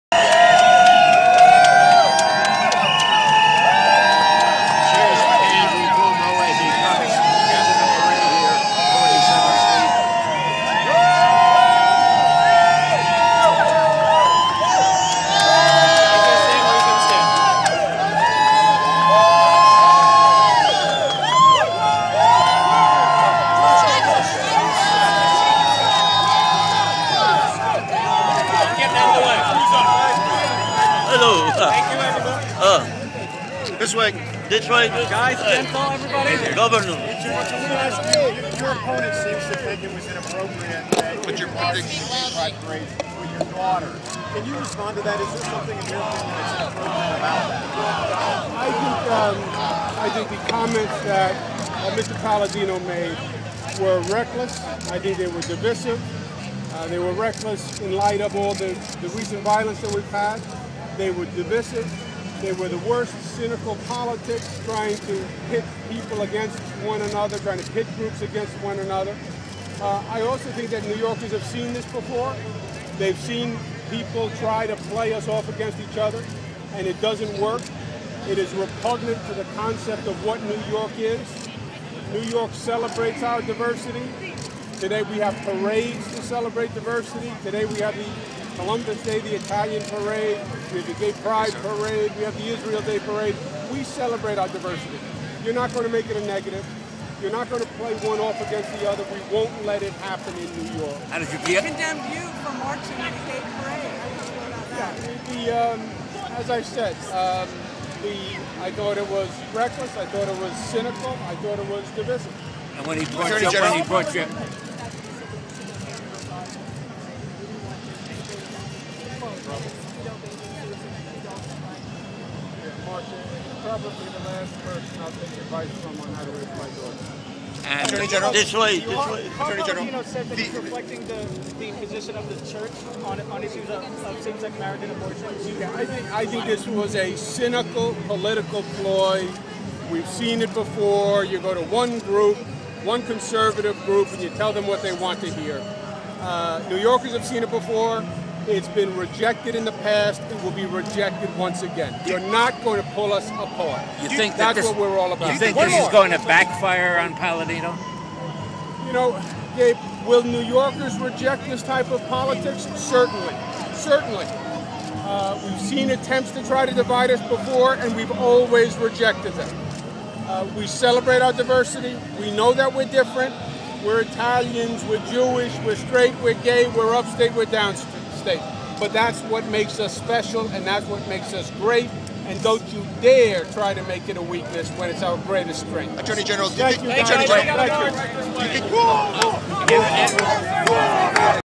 Attorney General and Candidate for New York Governor stated that Carl Paldino play a cinical political ploy by playing people against each other. Video by Rafael Martínez Alequín for yourfreepress.blogspot.com